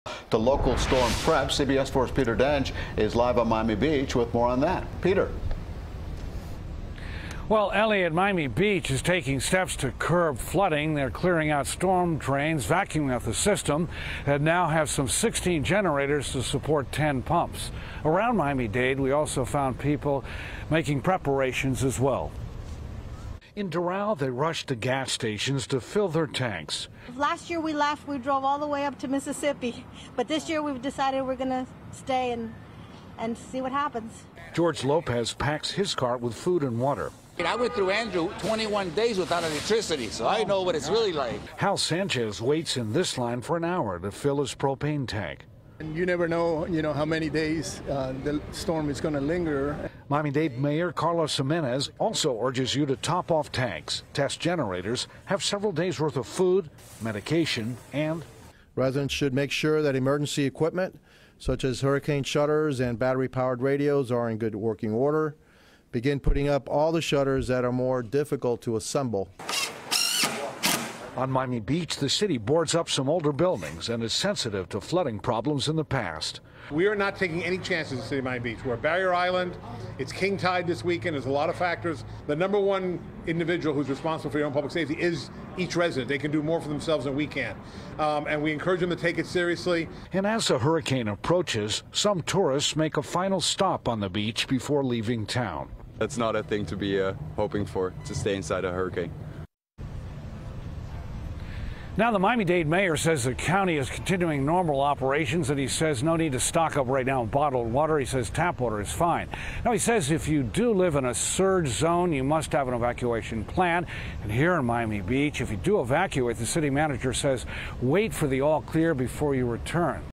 Miami-Dade Mayor Carlos Gimenez Urges Residents To Prepare Early
At a news conference Thursday morning, Miami-Dade Mayor Carlos Gimenez stressed the importance of residents to start checking your supplies, test your generators, get fuel for those generators and top off your vehicle’s gas tanks. You should also get your medication refills if necessary.